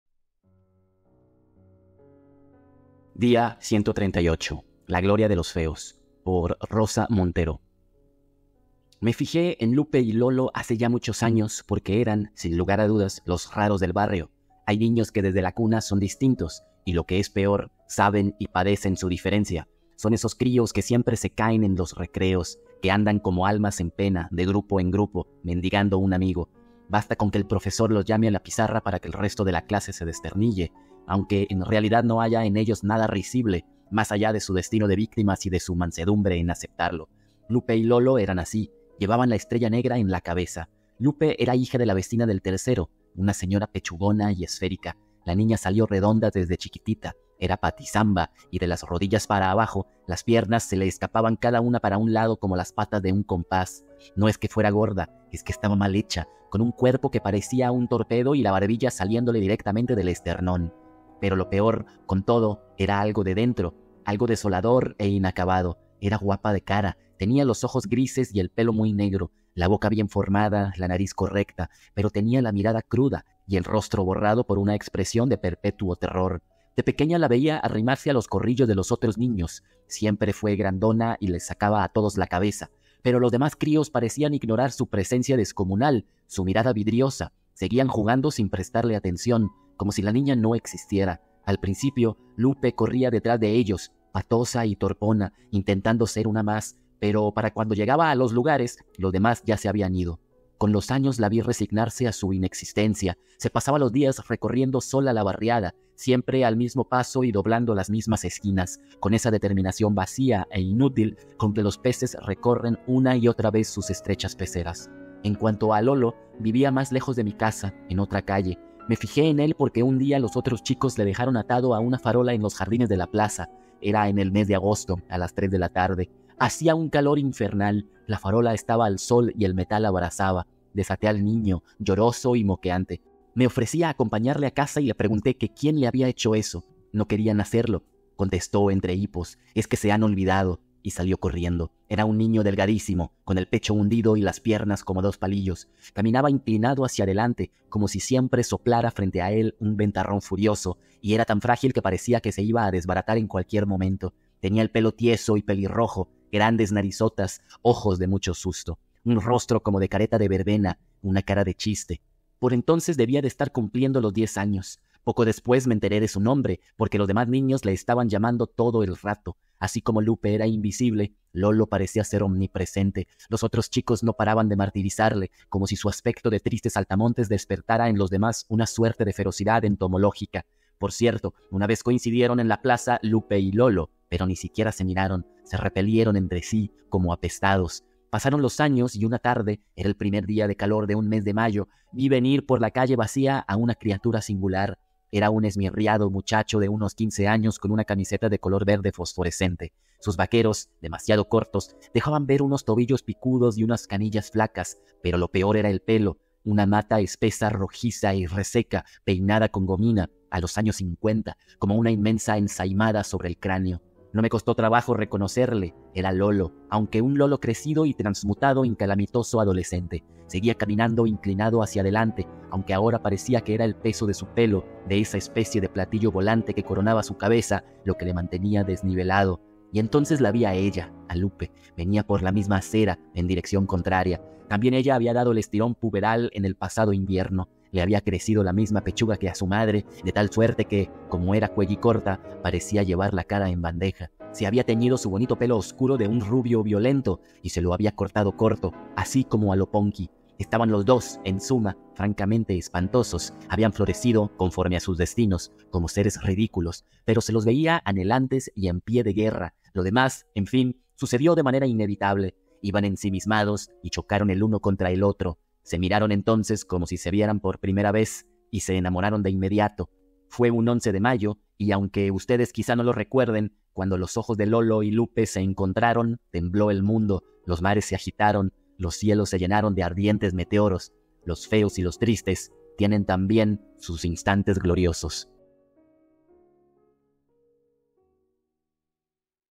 Día 138 - La gloria de los feos, por Rosa Montero
Literatura todos los días: un cuento cada 24 horas.

Hoy, día 138 - La gloria de los feos, por Rosa Montero

También dale "me gusta" y haz clic en la campanita para no que no te pierdas ningún cuento.